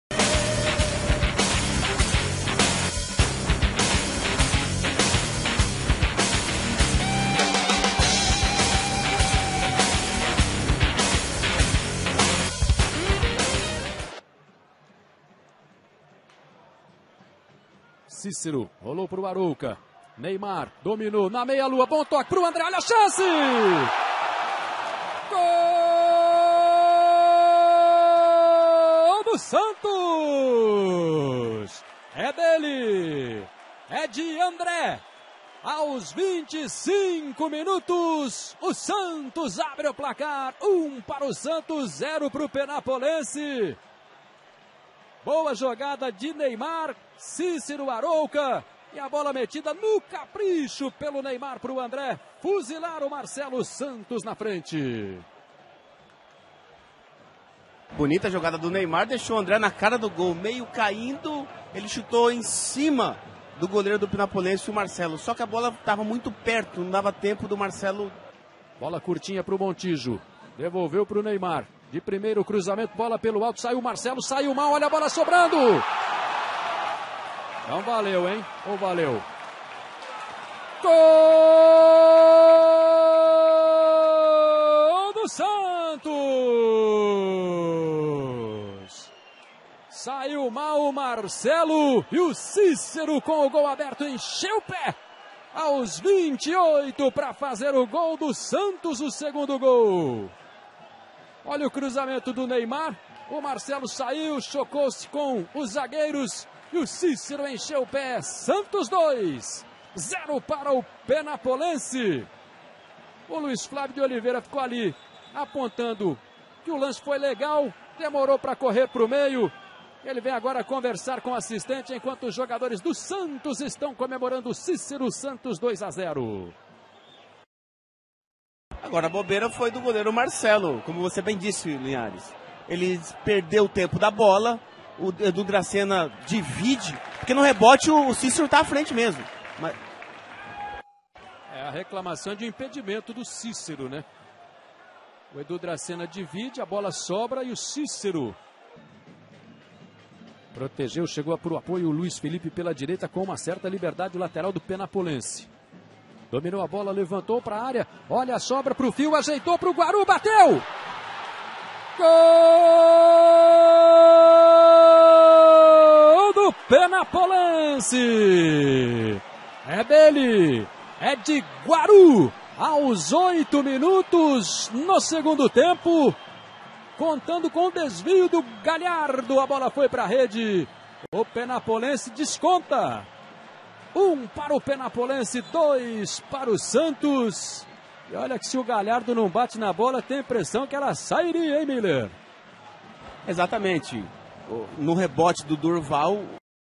Santos 2 x 1 Penapolense - Campeonato Paulista - Gols 21/04/2013
SANTOS VENCE PENAPOLENSE, E AMBOS SE CLASSIFICAM NA VILA BELMIRO
Peixe terá clássico com o Palmeiras nas quartas, e time do interior enfrentará o São Paulo. Jogo tem ritmo acelerado